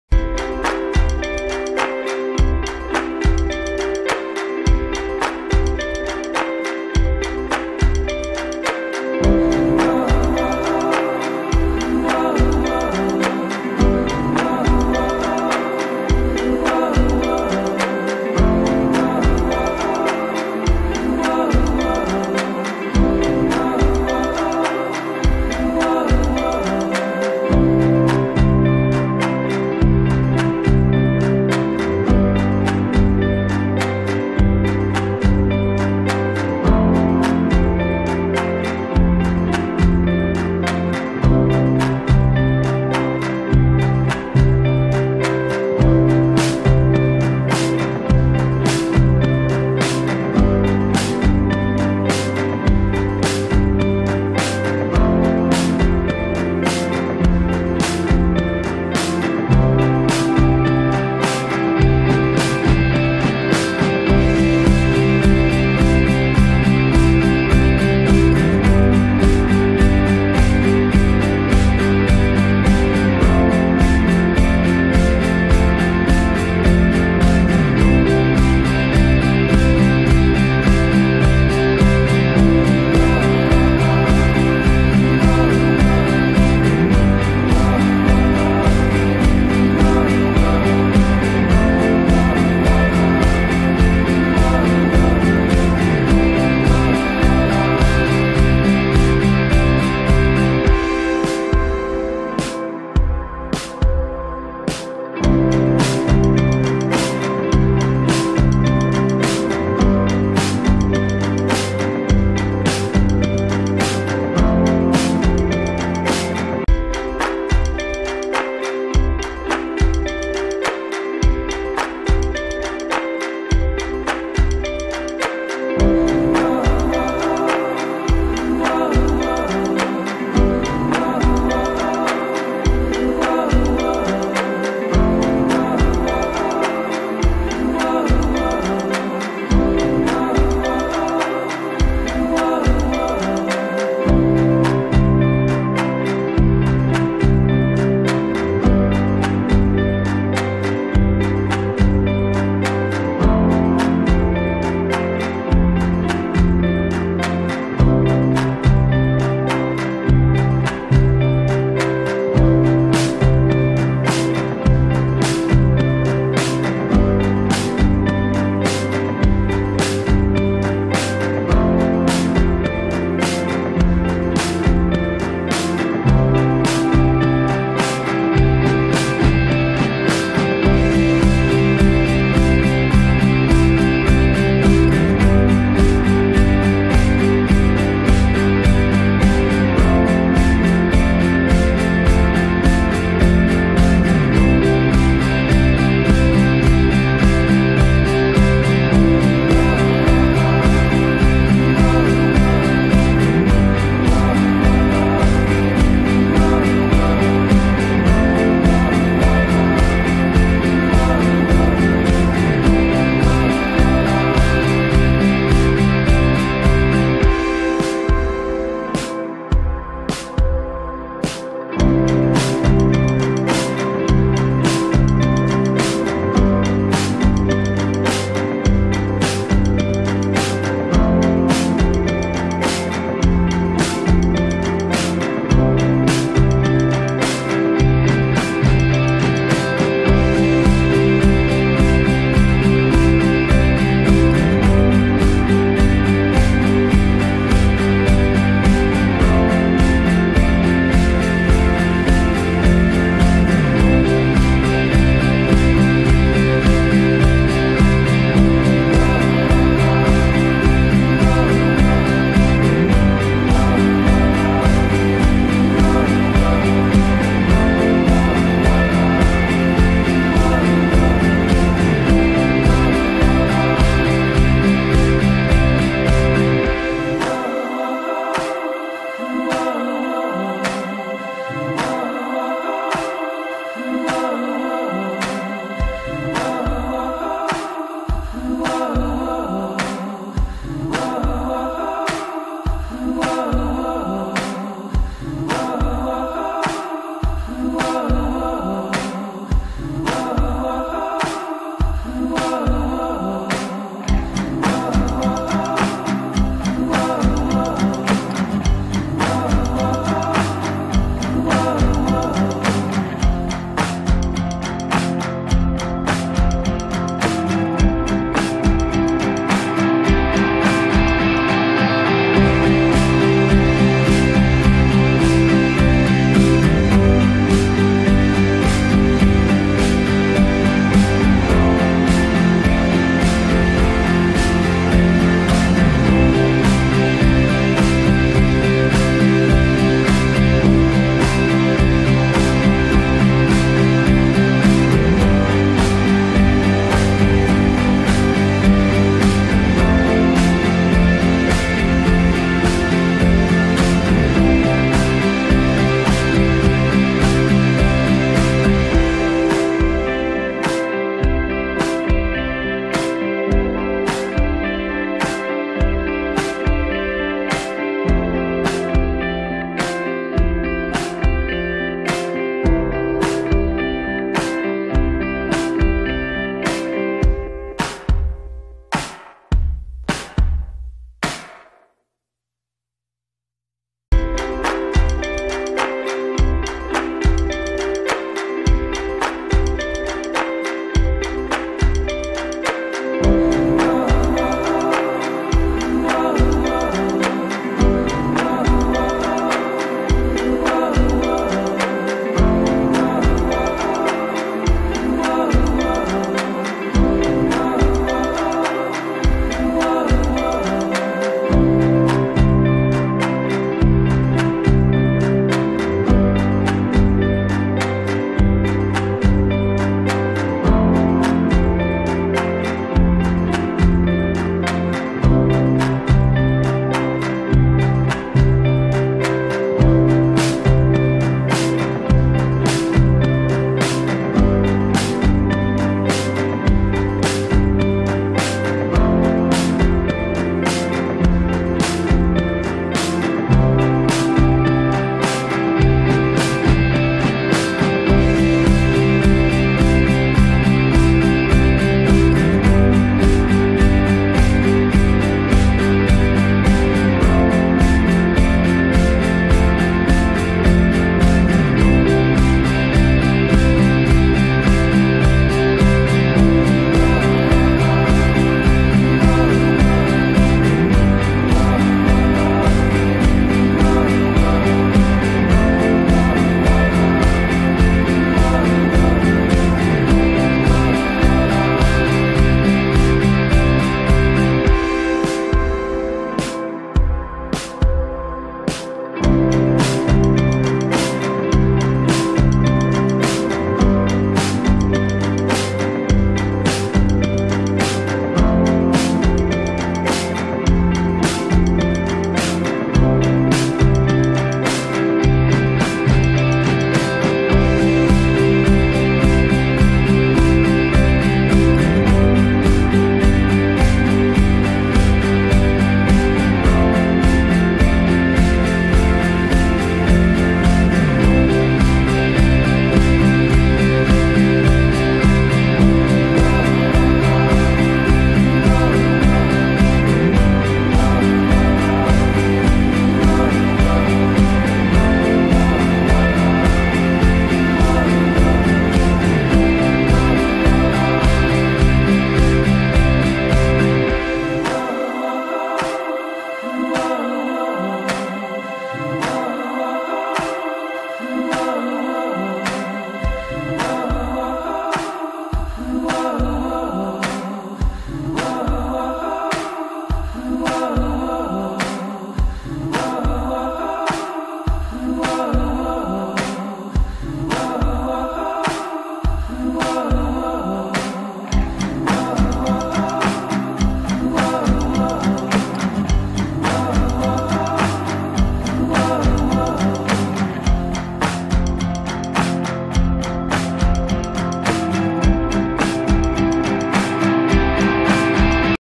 Mobile App Design splash screen design - In Photoshop
hello welcome in this video i am going to show you Andriod app Design splash screen in photoshop